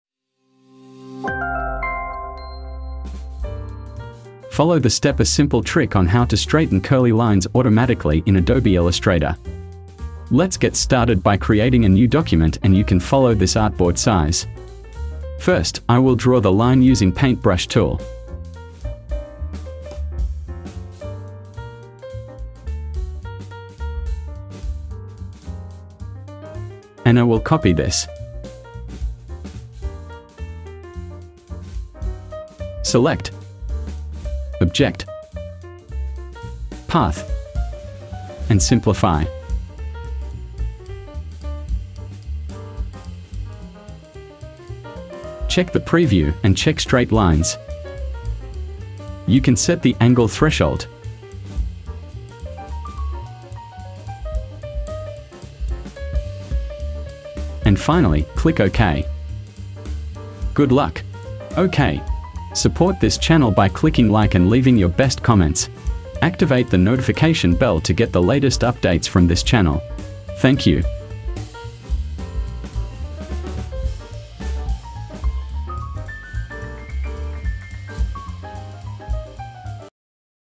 Trick to Straighten Curly Lines Automatically | Adobe Illustrator Tutorials
basic tips how to traighten curly lines automatically using simplify adobe illustrator.